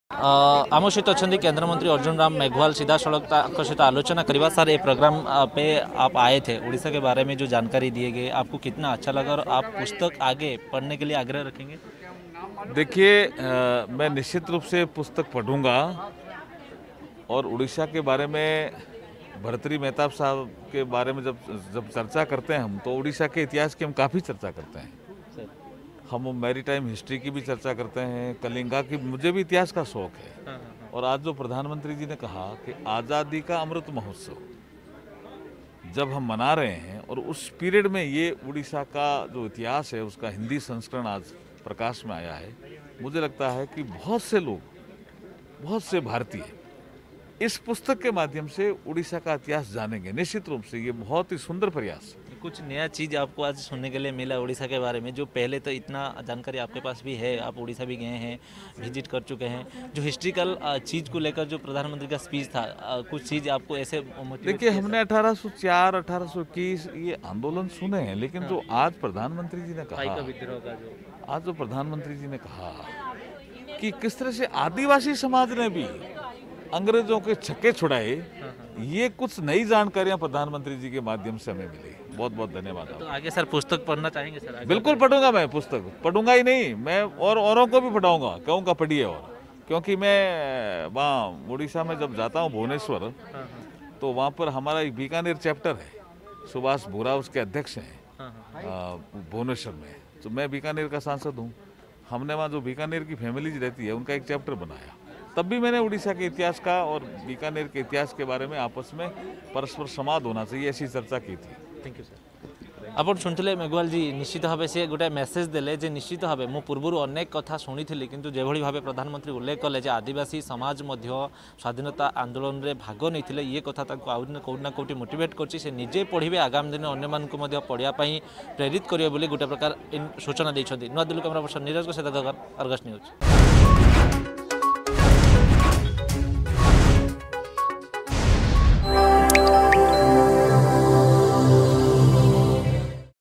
କେନ୍ଦ୍ରମନ୍ତ୍ରୀ ଅର୍ଜୁନ ରାମ ମେଘୁଆଲଙ୍କ ସହ ସ୍ବତନ୍ତ୍ର ଆଲୋଚନା
ଶୁଣନ୍ତୁ କଣ ପ୍ରତିକ୍ରିୟା ରଖିଛନ୍ତି କେନ୍ଦ୍ରମନ୍ତ୍ରୀ...